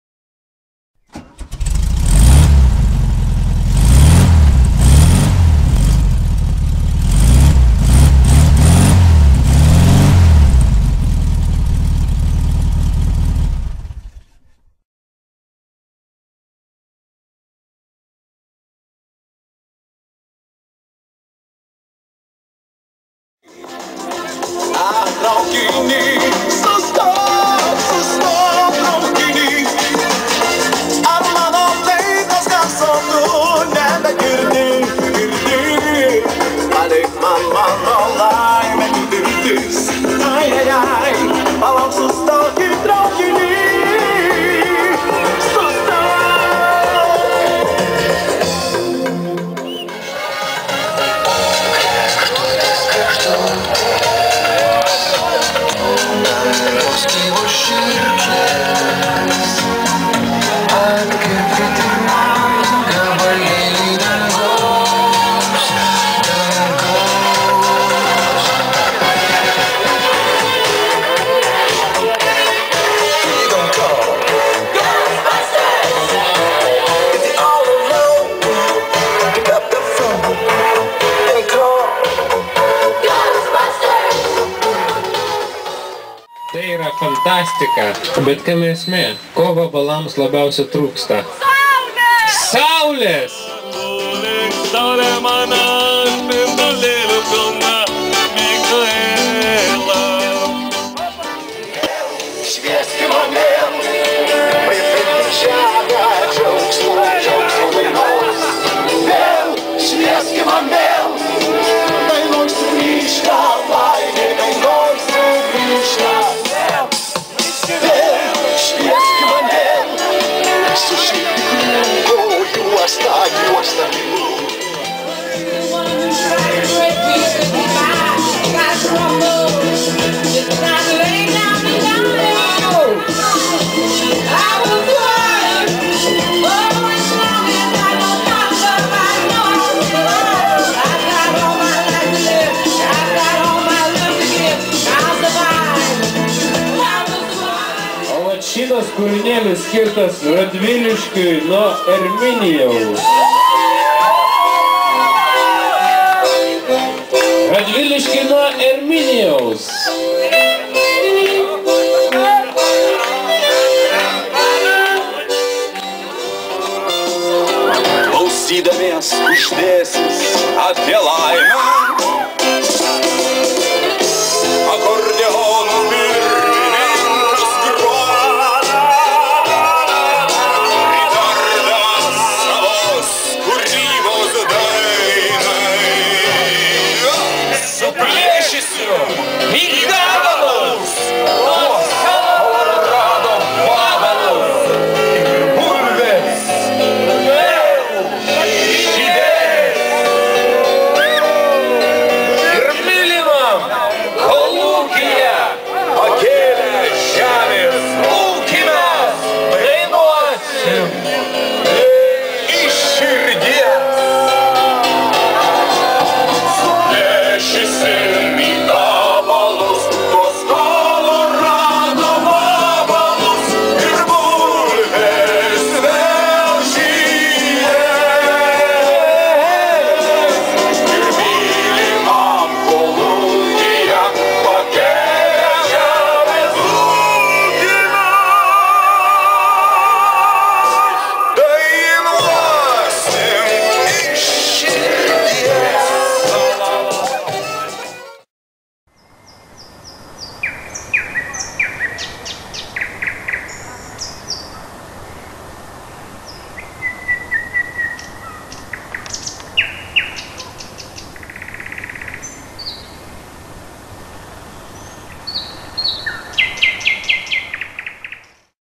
Smalininkai - Vabalų sąskrydis 2012 (pašėlusi šeštadienio naktis)
Smalininkai, Pašvenčio girininkija, Vabalų šventės šeštadienio nakties linksmybių akimirkos. Nors tai vasaros pradžia, bet oras buvo nepakartojamas :)